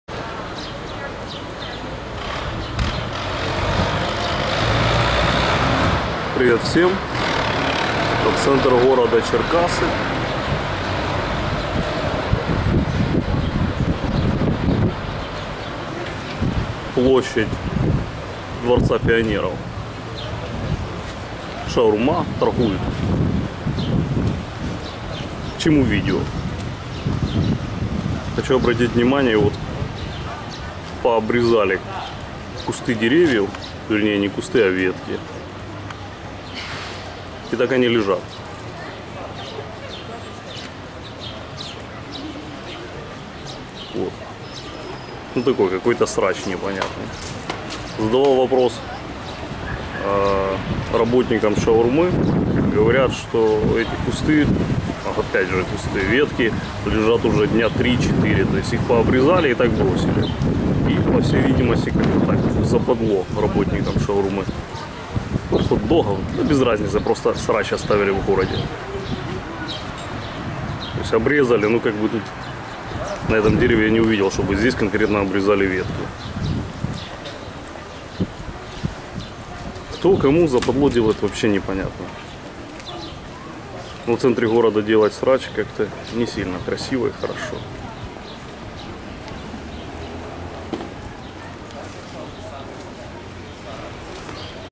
В Центре Черкасс развели срач возле Дворца Пионеров
21 мая, в день когда в городе проходят "Мегамарш Вишиванок" RockFlyFestCherkassy и другие чудесные мероприятия в центре вот такие кучи веток с деревьев. Если вы обратили внимания то, что там даже нет асфальта хотя еще пол года назад он был. Поговаривают, что его убрали чтобы насолить владельцам киосков с едой быстрого приготовления.